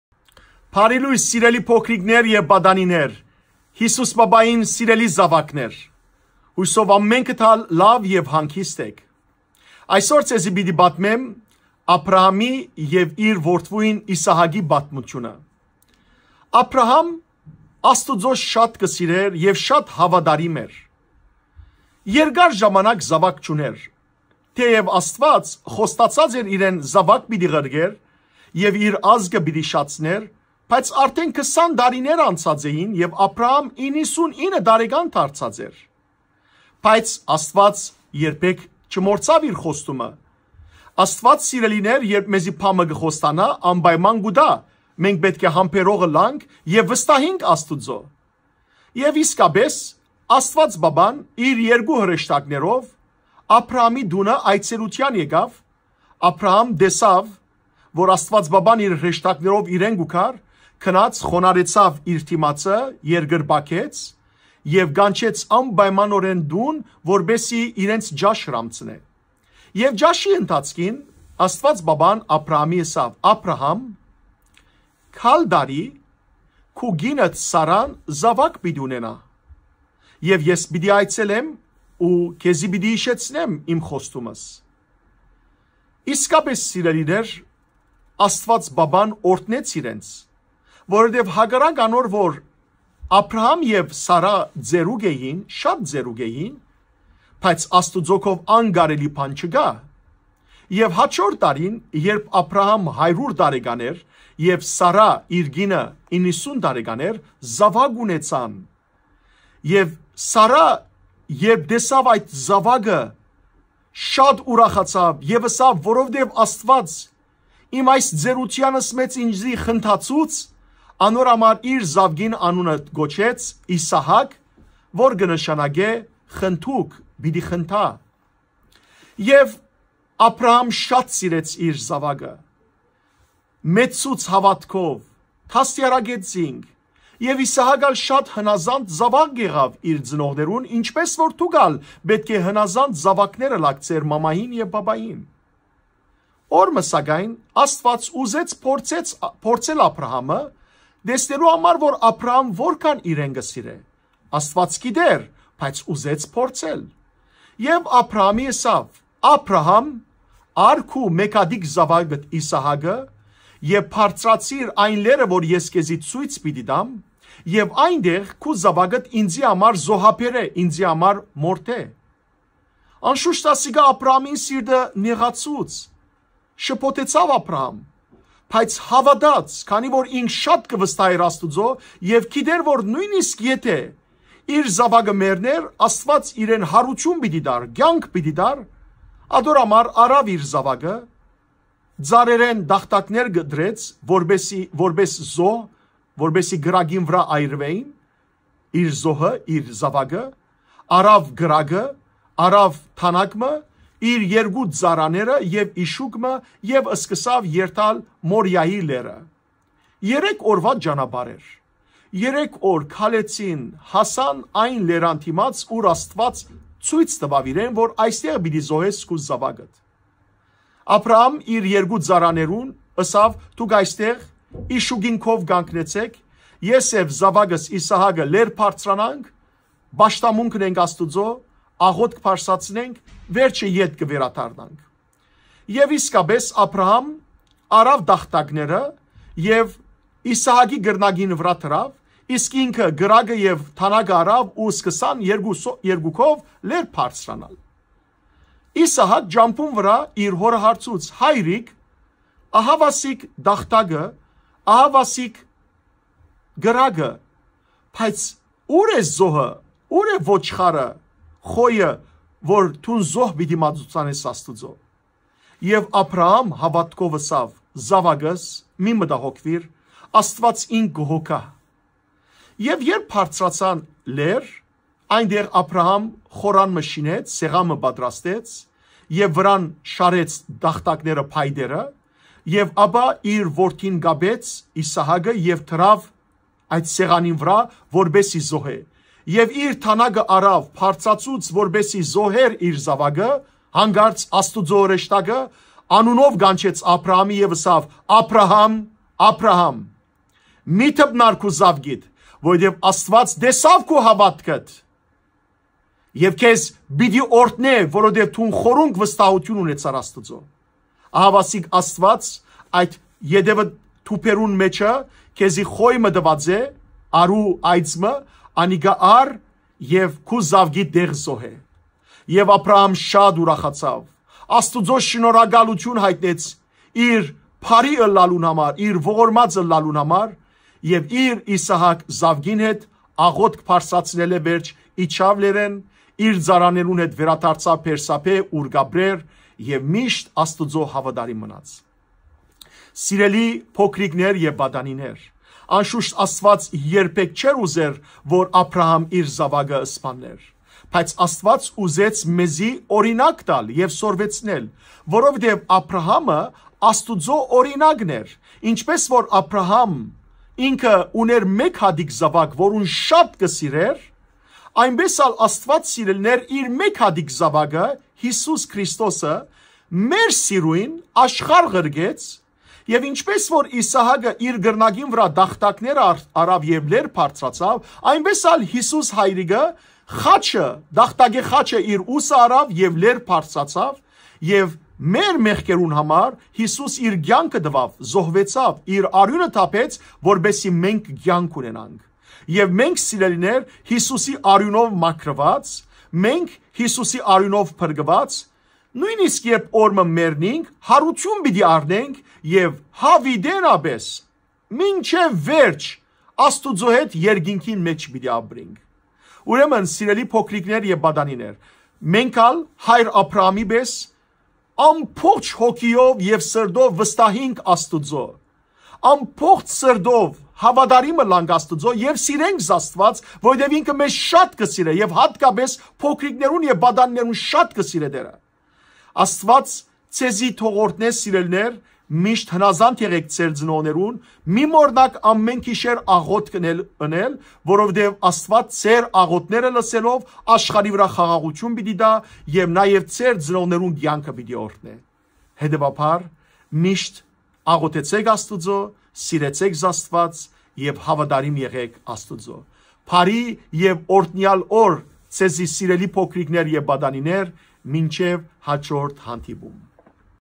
Աբրահամ կը պատրաստուի Աստուծոյ զոհաբերել Իսահակը (պատմուած փոքրերու համար) (Ապրիլ/April 5, 2019)
Աբրահամ կը պատրաստուի Աստուծոյ զոհաբերել Իսահակը (պատմուած փոքրերու եւ պատանիներու  համար) (Ապրիլ/April 5, 2019)